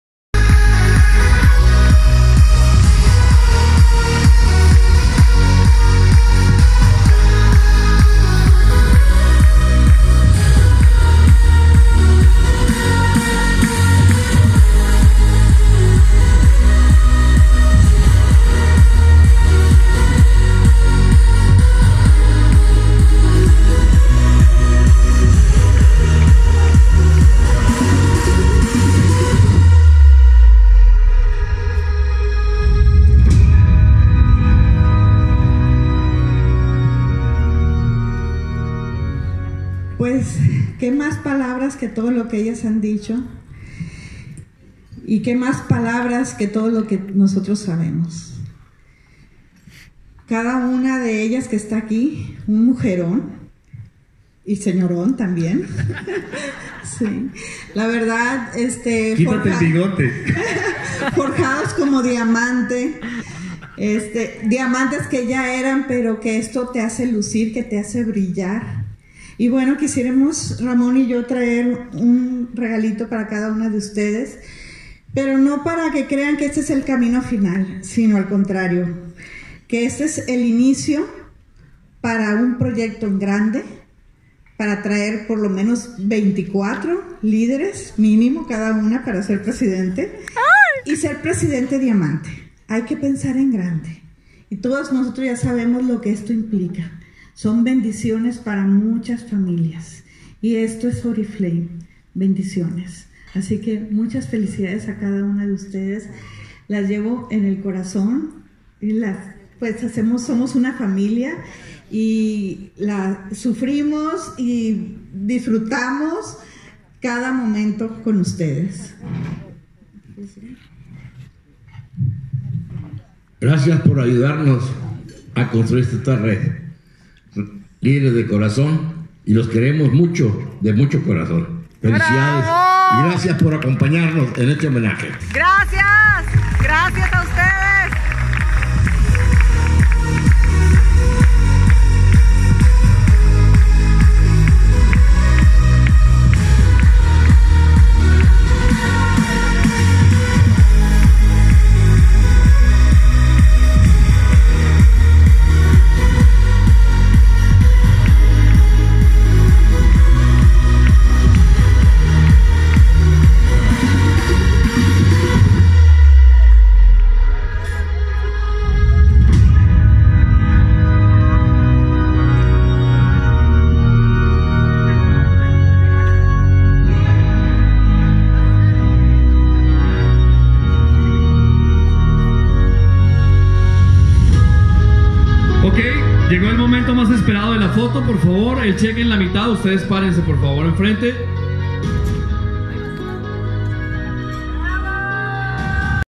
Continúa reconocimiento Lety en el Castillo de Viñuelas Madrid España
Reconocimiento de Lety líder Diamante Oriflame en España Conferencia Oro 2018